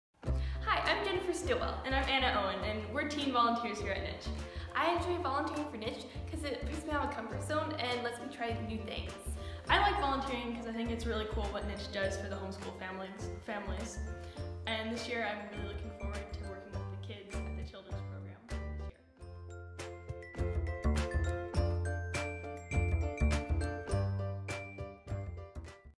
Teen Volunteer Opportunities at the Homeschool Iowa Conference
Is your homeschooling teen looking for an opportunity to join other home educated students in fellowship and service?

Our Homeschool Iowa Teen Volunteer Team is the answer!

We're looking for willing young people with the heart to serve. Volunteer positions like this look great on a resume or transcript, and the experience is invaluable.